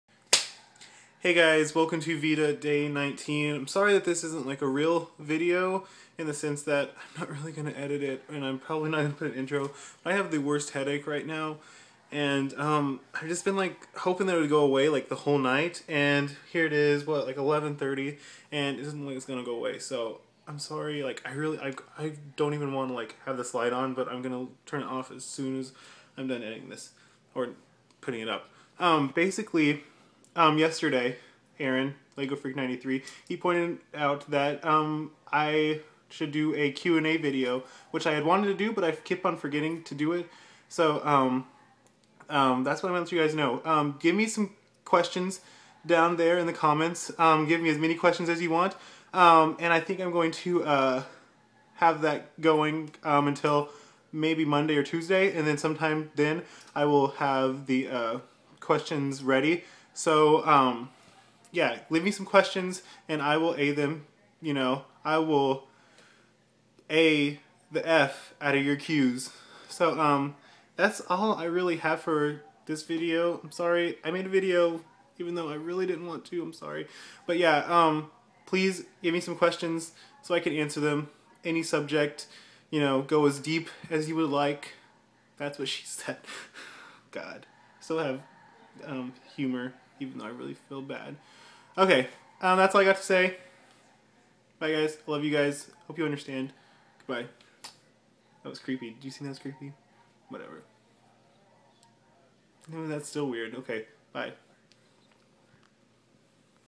VEDA 2011 Day 19: A me some Q's
Sorry it's not a legit video. Please ask me some questions :)